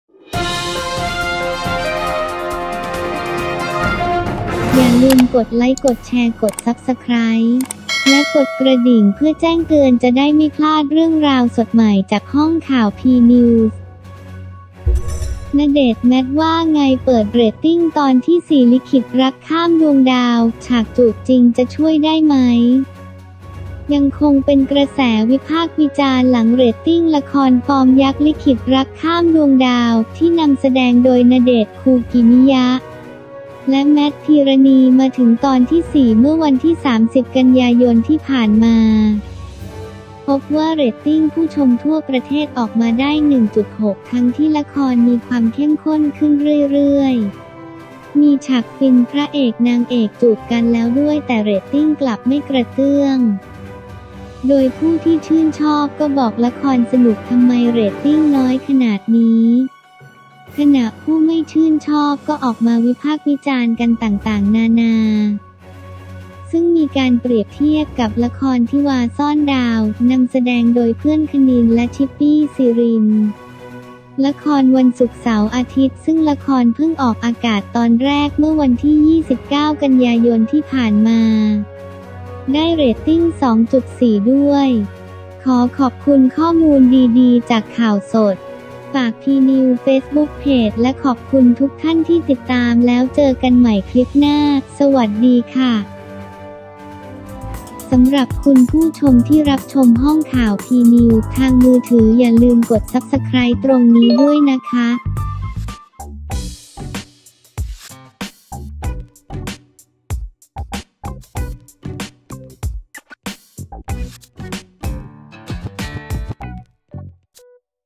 ณเดชน์-แมท ว่าไง เปิดเรตติ้ง Ep.4 ลิขิตรักข้ามดวงดาว ฉากจูบจริง จะช่วยได้มั้ย!
ณเดชน์-แมท ว่าไง เปิดเรตติ้ง ลิขิตรักข้ามดวงดาวep4 ฉากจูบจริง จะช่วยได้มั้ย
จูบจริงสุดฟิน – ยังคงเป็นกระแสวิพากษ์วิจารณ์ หลังเรตติ้ง ละครฟอร์มยักษ์ ลิขิตรักข้ามดวงดาว ที่นำแสดงโดย ณเดชน์ คูกิมิยะ และ แมทภีรนีย์ คงไทย มาถึง Ep.4 เมื่อวันที่ 30 ก.ย. ที่ผ่านมา

พบว่า เรตติ้งผู้ชมทั่วประเทศ ออกมา ได้ 1.6 ทั้งที่ละครมีความเข้มข้นขึ้นเรื่อยๆ มีฉากฟิน พระเอก-นางเอก จูบกันแล้วด้วย แต่เรตติ้งกลับไม่กระเตื้อง

โดยผู้ที่ชื่นชอบก็บอกละครสนุก ทำไมเรตติ้งน้อยขนาดนี้ ขณะผู้ไม่ชื่นชอบ ก็ออกมาวิพากษ์วิจารณ์กันต่างๆ นานา

ซึ่งมีการเปรียบเทียบกับ ละคร ทิวาซ่อนดาว นำแสดงโดย เพื่อน คณิน และ ชิปปี้ ศิรินทร์ ละครวันศุกร์-เสาร์-อาทิตย์ ซึ่งละครเพิ่งออกอากาศตอนแรก เมื่อวันที่ 29 ก.ย. ที่ผ่านมา ได้เรตติ้ง 2.4 ด้วย

ขอขอบคุณข้อมูลดีดีจาก ข่าวสด